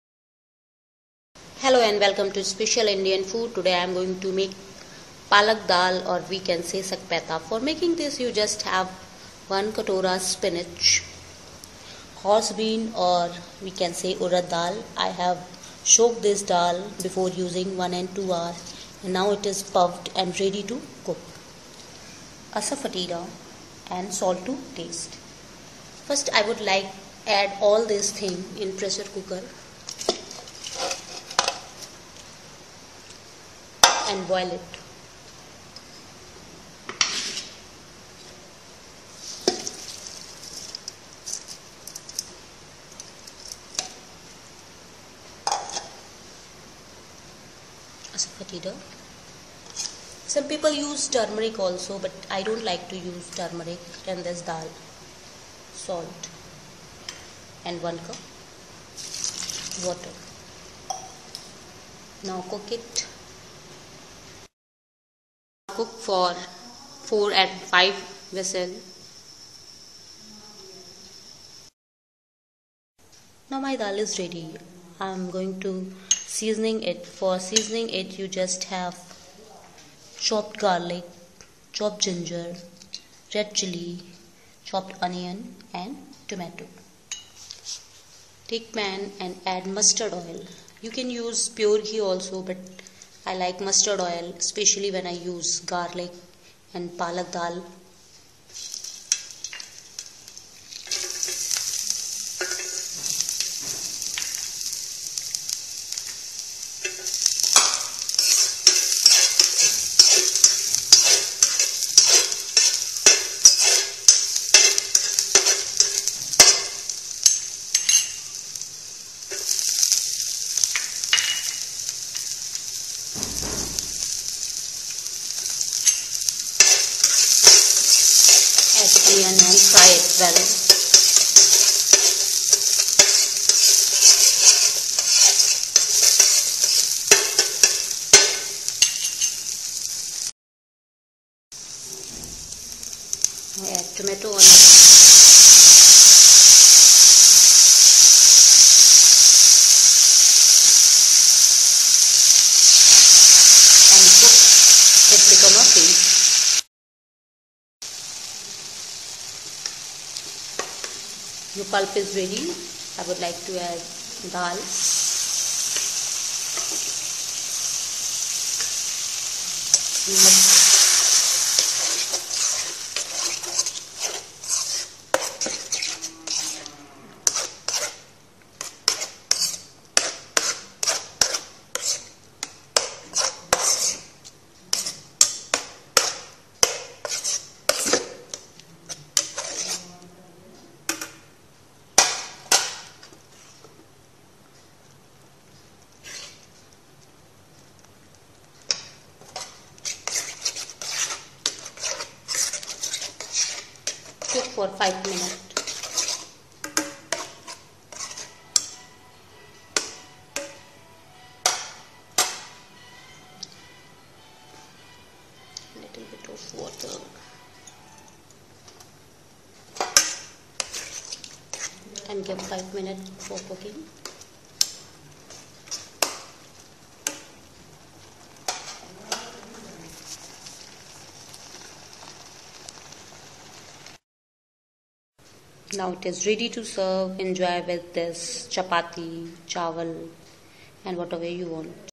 URAD SPINACH DAAL(HEALTHY DISH)
Palak daal is made by black lentil,spinach,turmeric powder,ginger,garlic